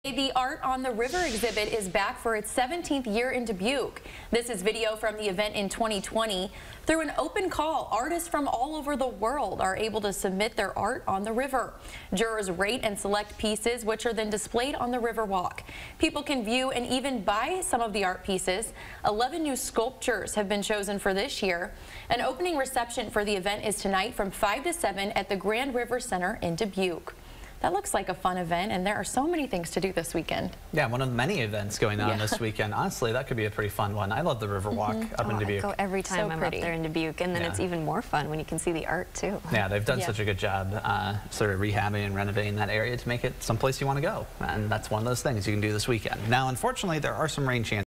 Art on the River exhibit returns for 17th year in Dubuque
The Art on the River exhibit is back for its 17th year in Dubuque on Friday.